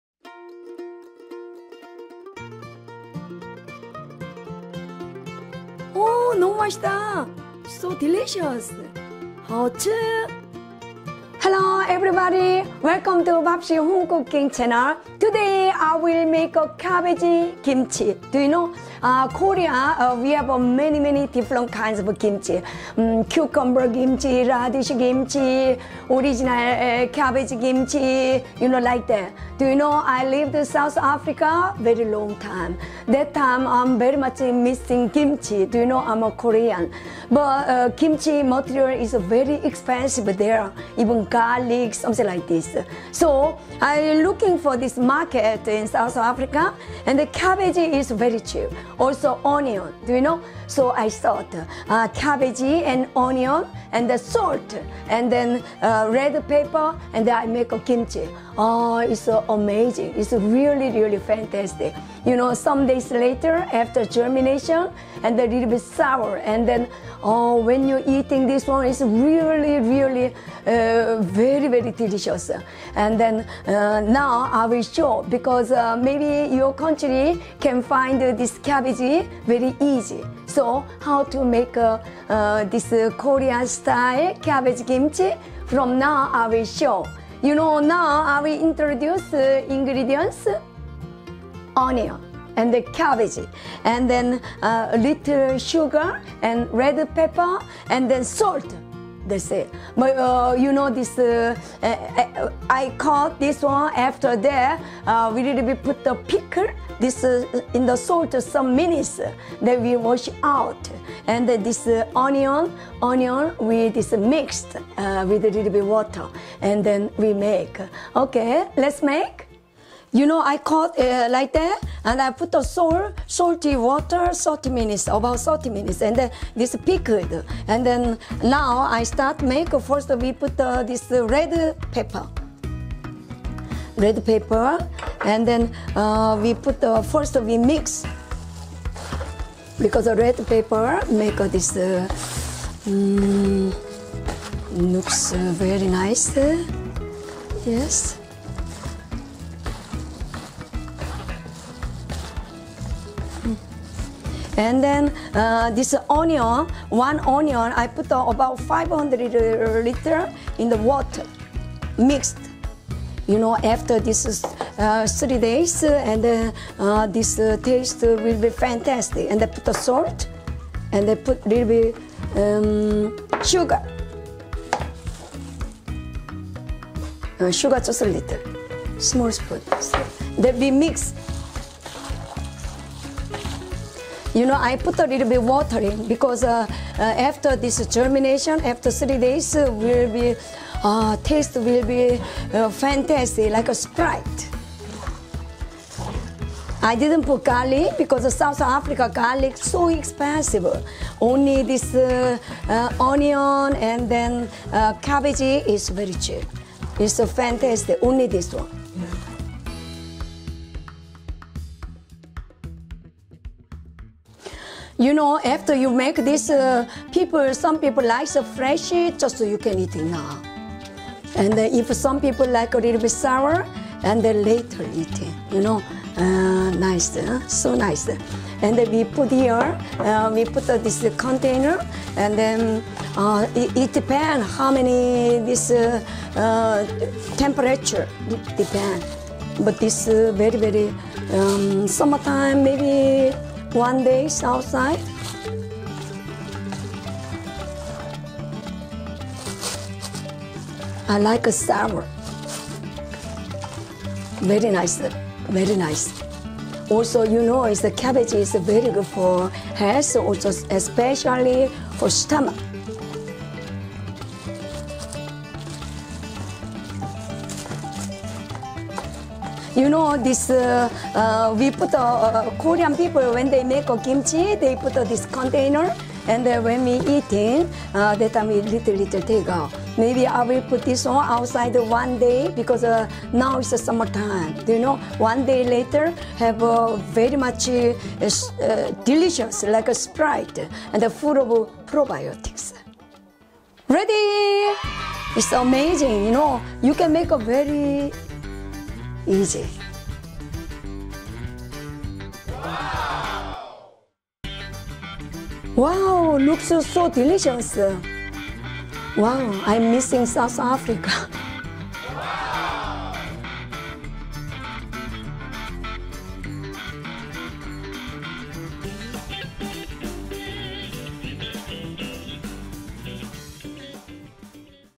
[ENG] Easy Korean Cabbage Kimchi, 양배추김치, How to make Kimchi if you don't have napa cabbage?
Hi Everyone!
Today I will show you how to make Korean Style Fresh Cabbage Kimchi!
It is called "Yangbaechu Kimchi" (Cabbage Kimchi) in Korean.
There are many types of Kimchi in Korea, we usually use many kinds of vegetables.
This Kimchi is made with cabbage!
You can try it if you don't have napa cabbage in your country!
The ingredients are very easy to find, so you don't have to worry about making Kimchi in your country!
The Recipe is very simple and it is very tasty and fresh!
Please just try it!

[Ingredients]
Salted Cabbage
(Cut 1 Cabbage into pieces. Wash them and put 150g Salt for 30 minutes. And wash the salted Cabbage with fresh water.)
Onion 1  (Grind the Onion in a blender with Water 500ml)
Red Pepper Powder 2 tbsp
초간단 양배추김치 레시피! 아프리카에서 만들어 먹던 눈물의 요리 양배추 김치! 액젓없이 간단하게 양배추 김치 담그는 법

이번영상에선 정말 간단하게 뚝딱 만들 수 있는 아삭아삭 신선하고 맛있는 양배추김치를 소개해드리려고 해요!
양배추김치는 제가 사우스 아프리카 (남아공)에서 한국요리가 그리워 만들어 먹던 눈물의 요리랍니다!
한식 재료가 부족한 해외에서 간단하게 만들어 먹을 수 있게 레시피를 만들어 보았어요!
매콤하게 잘 익은 김치를 냉장고에 넣어 시원하게 해서 먹으면 밥 한그릇 뚝딱, 더운 여름 입맛을 돋우는 별미랍니다!
여러분도 꼭 한번 드셔보세요!

[재료]
절인 양배추 (중간 크기 양배추 1개에 소금150g을 넣어 30분 절인 후 물에 깨끗이 씻어줍니다.)
양파 1개 (양파 1개와 물 500ml를 믹서에 함께 갈아줍니다.)
소금 1 큰술
설탕 3 작은술
고춧가루 2 큰술